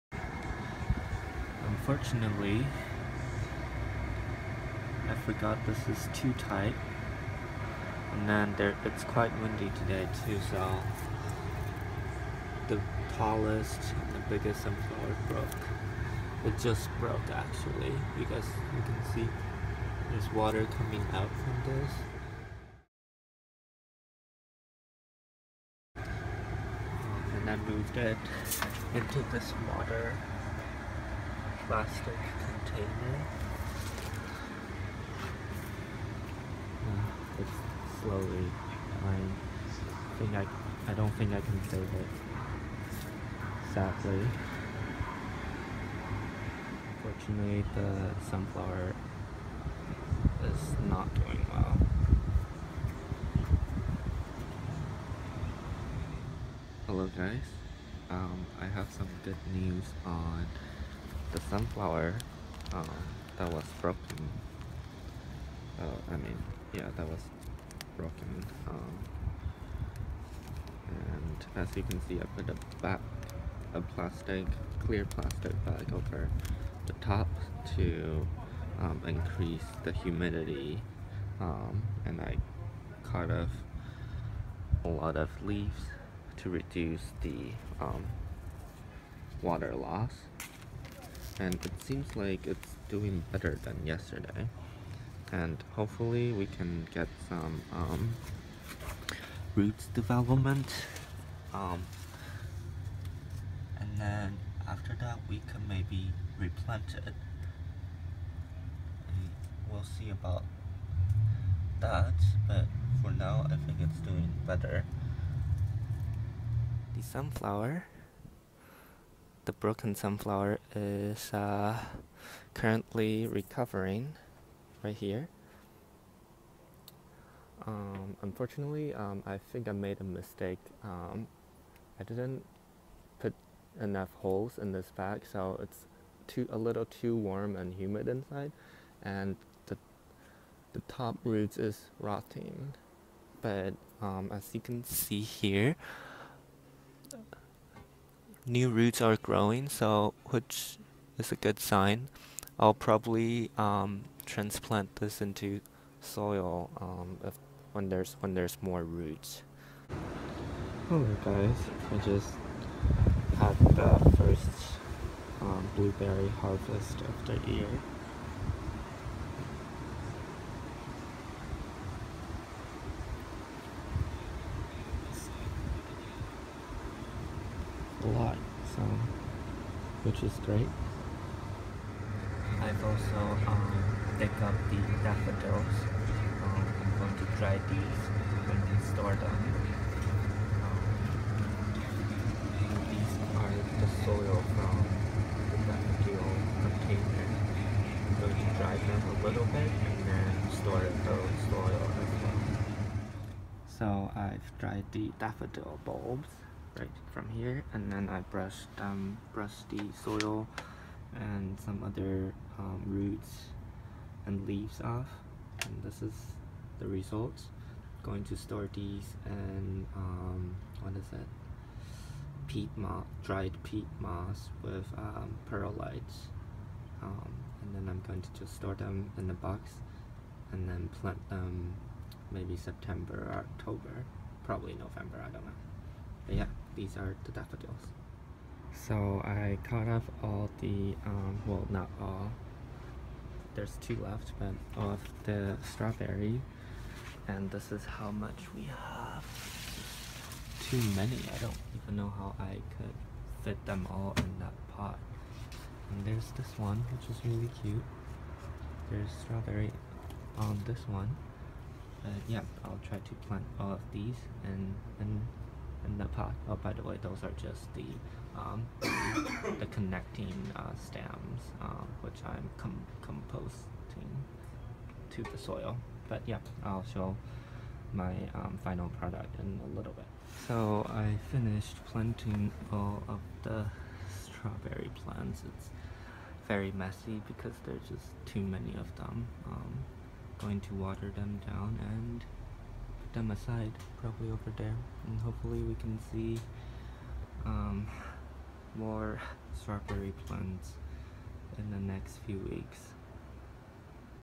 Garden Projects (July) - Sunflower, blueberry, daffodil, and strawberry
These is a few projects I've been working on in the garden.
1.Sunflower plant broke on a windy day so I wanted to fix and revive it.
2. Blueberry harvest
3. Daffodil bulb storage
4. Strawberry propagation